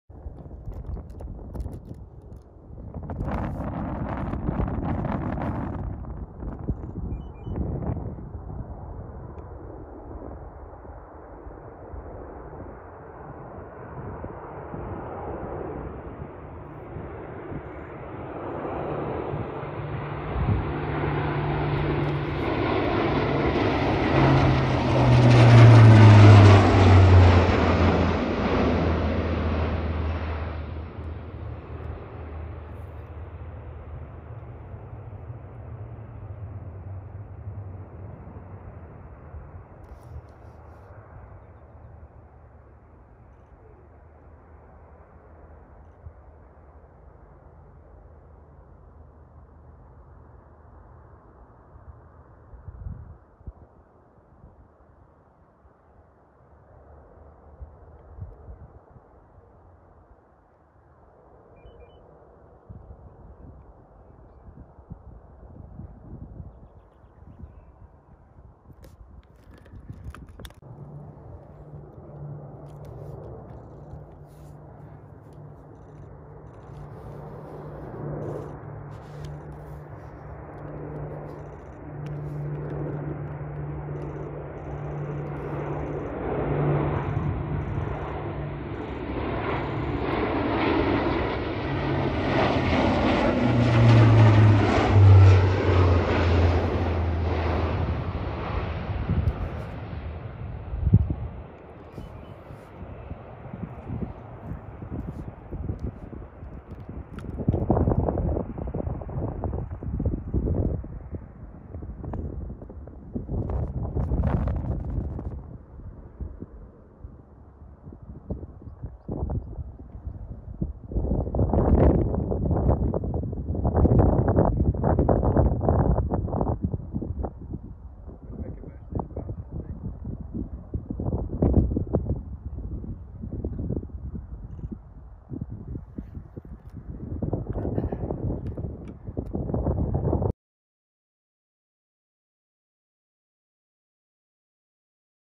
C130 Hercules in LFA7
RAF C130 Hercules passing Pulpit in The Mach Loop LFA7.